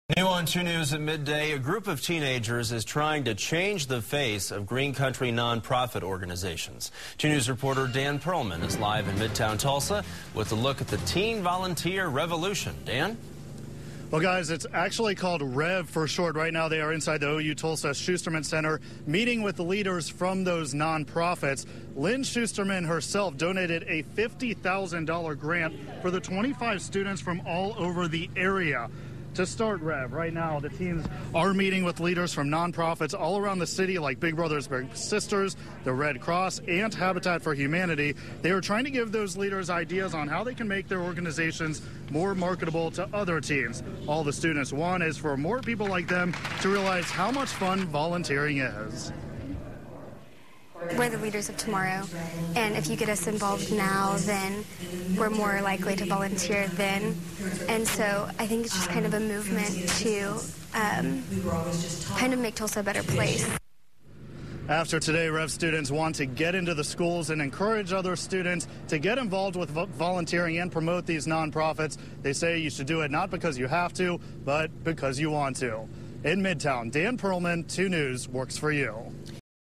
Teen Volunteers
25 teens meet with non-profit leaders to discuss volunteering